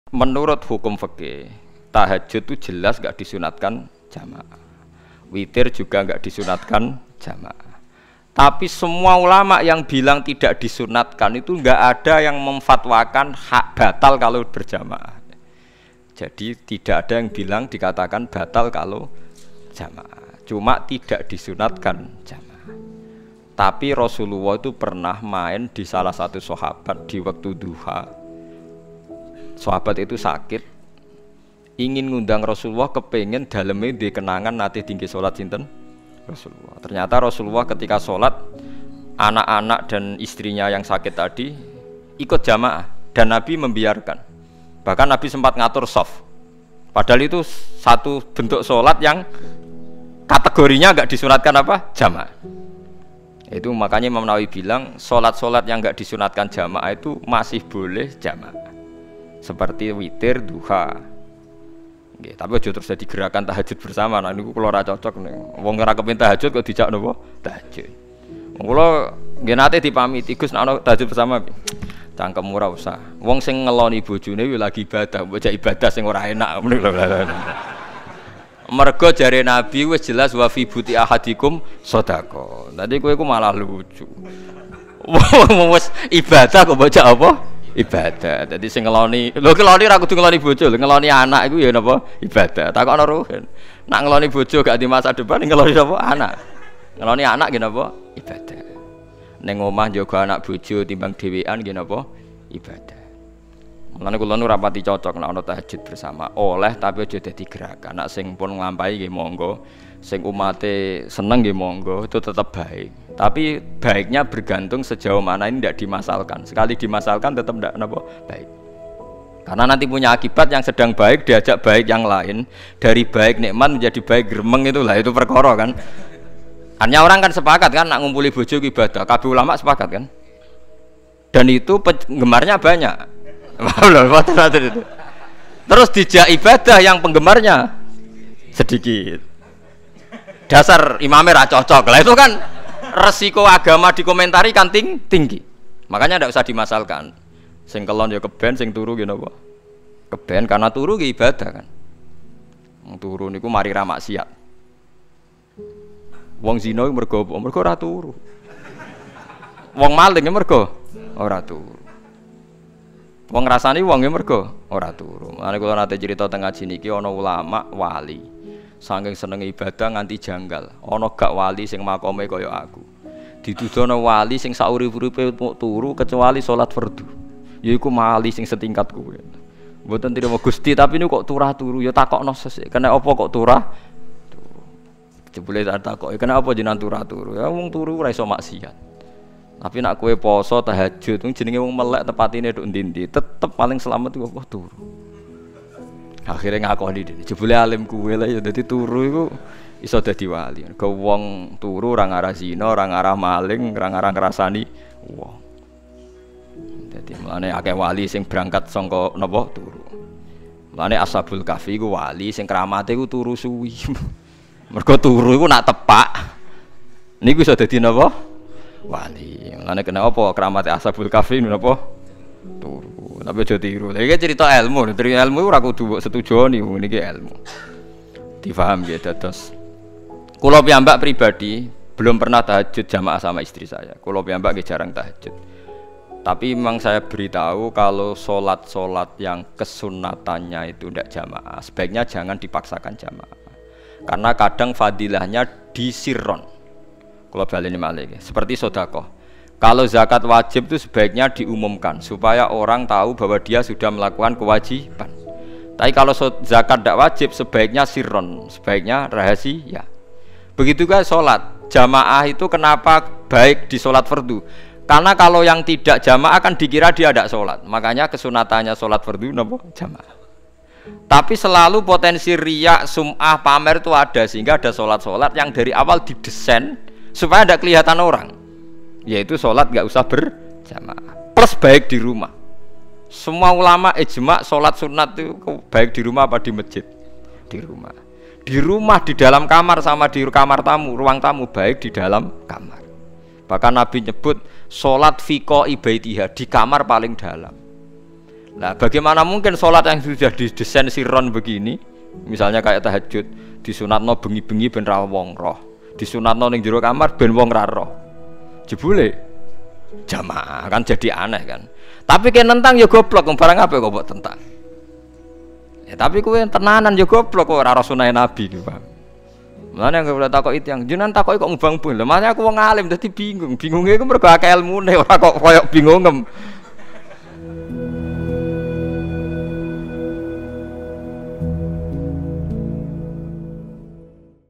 Nabi Pernah Salat Dluha Berjamaah - Gus Baha
Apakah salat sunah, seperti Dluha, boleh dilakukan secara berjamaah? Nabi pernah melakukannya! Waktu itu ada sahabat yang ingin rumahnya didatangi nabi, dan Kanjeng Nabi melakukan salat sunah di sana, bahkan sempat mengatur shaf!